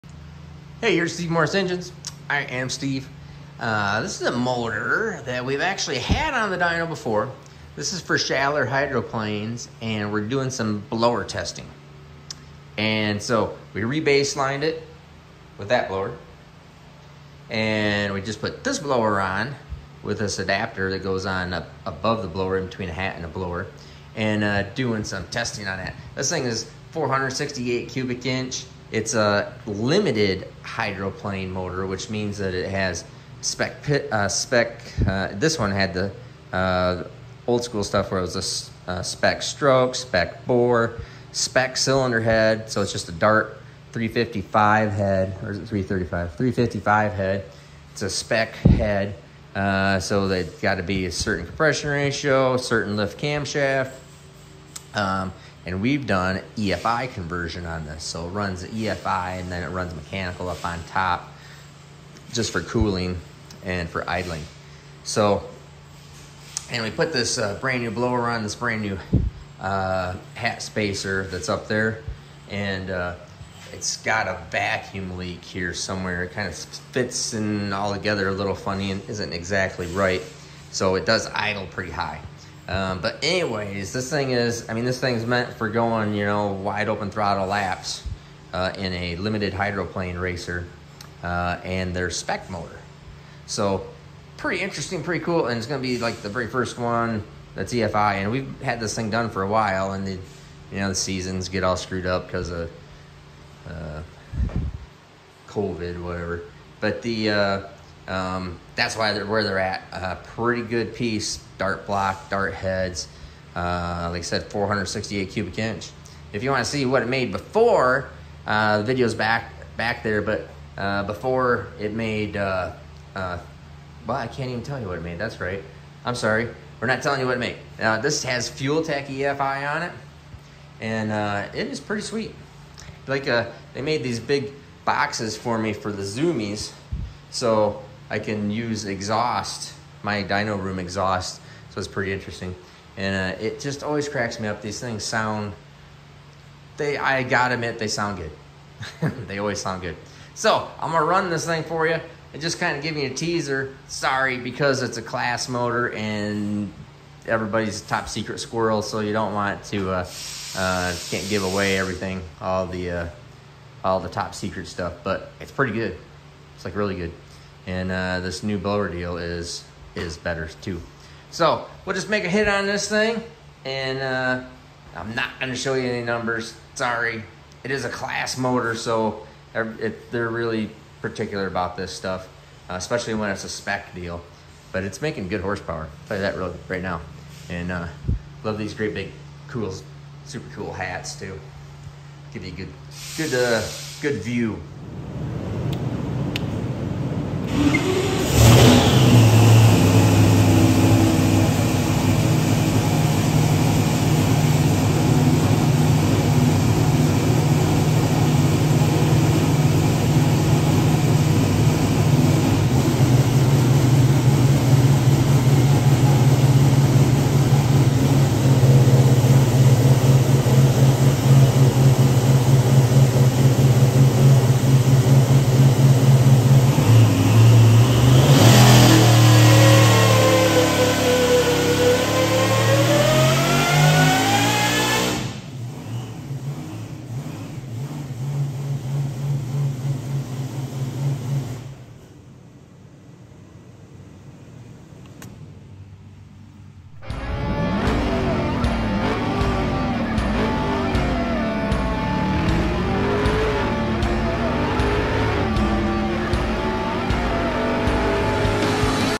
Supercool Limited Hydro Plane Dyno!
Check out the Limited Hydroplane race engine we build and are doing some blower testing on.
Sorry, this is a class engine and super top secret deal. So there is no power numbers.